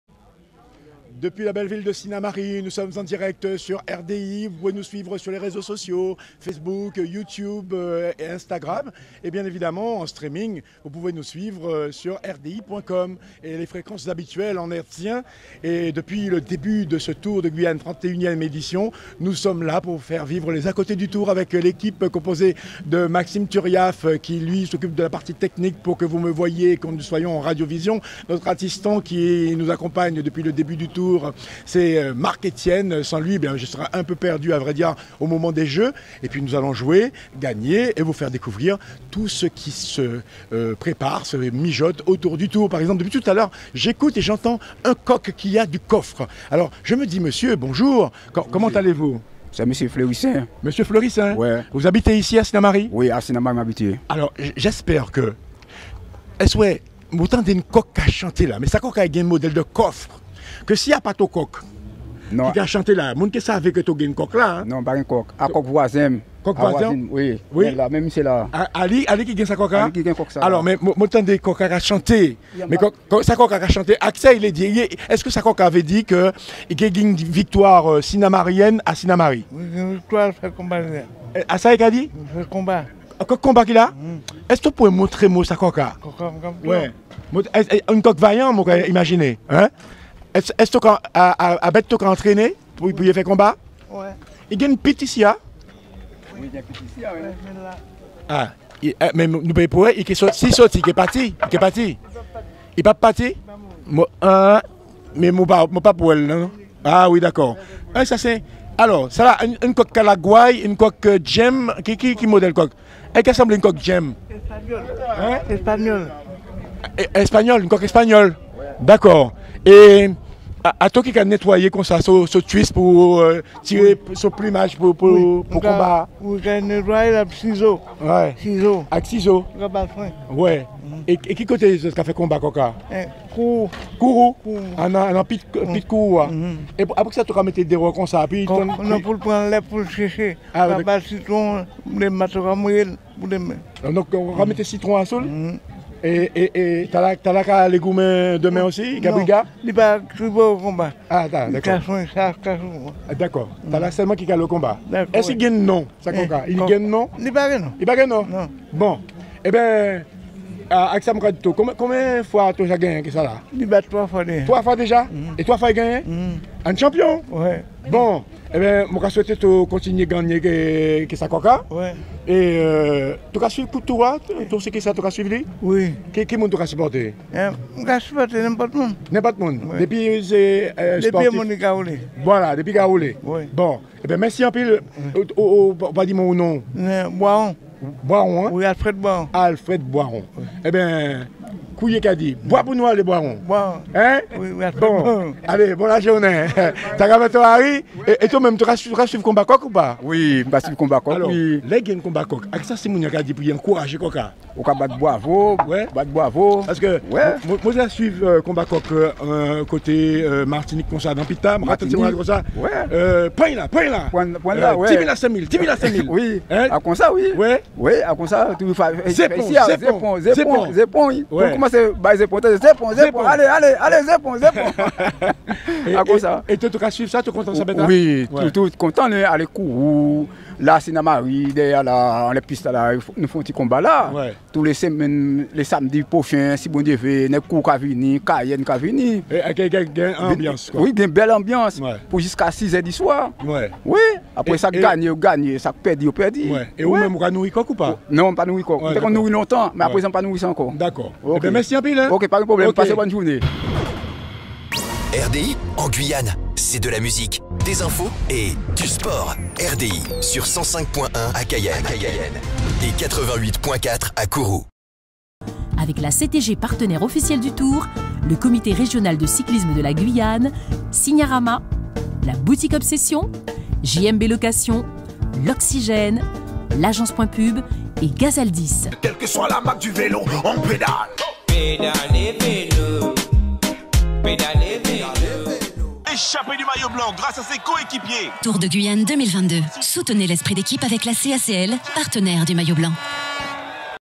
À qui le tour ? - Eleveur de coq Pitt story - TDG2022 étape 6 Saint-Laurent/Sinnamary
A Sinnamary Flyy va à la rencontre des supporters pour cette 6ieme étape Saint-Laurent / Sinnamary du Tour de Guyane 2022.